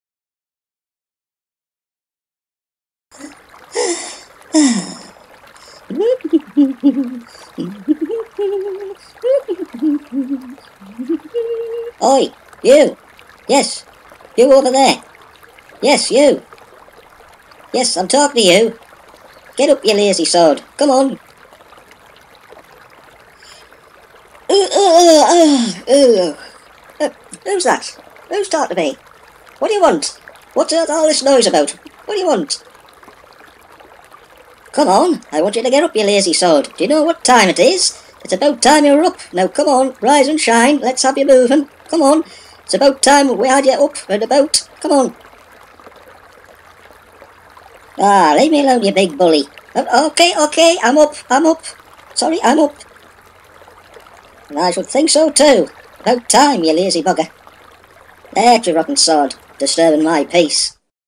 Lazy Sea Otter
Lazy Sea Otter just won't get up. :-)
Credits: Inspector J stream, courtesy of Freesound.org